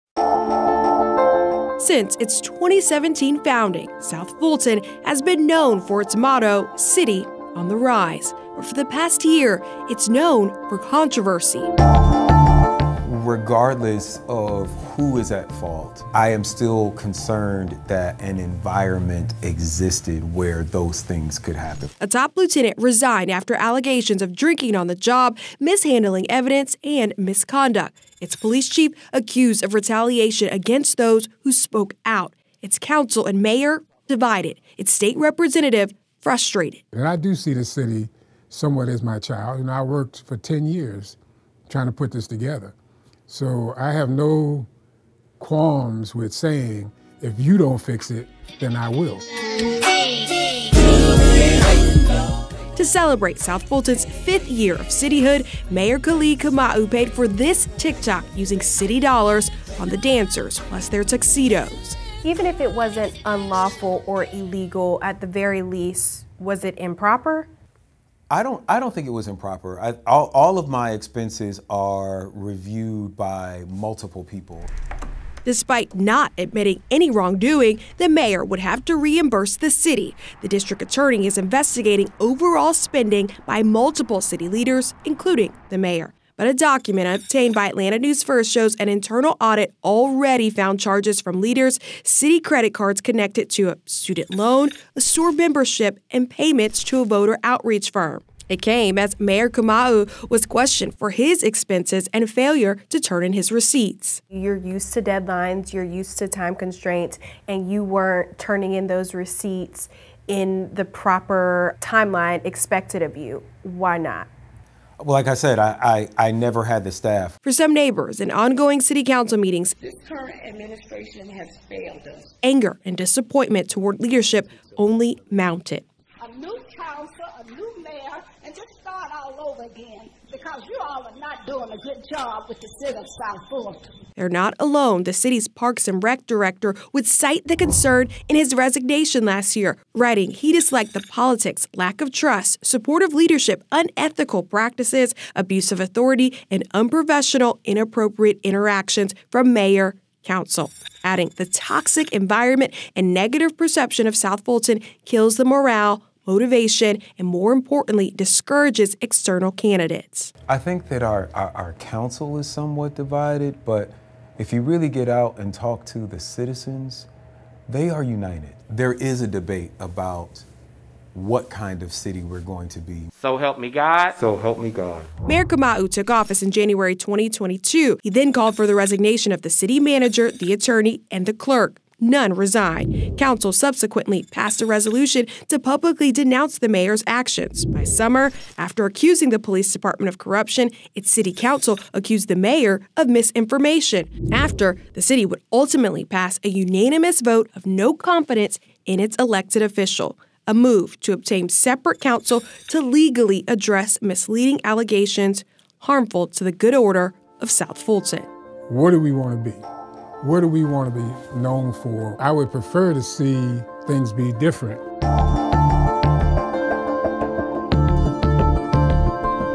South Fulton - Part 2: Lawmaker’s warning to South Fulton: ‘If you don’t fix it, then I will’
Since its 2017 founding, its motto has been “A City on the Rise.” But for the past year, it’s been known for controversy. February 27, 2023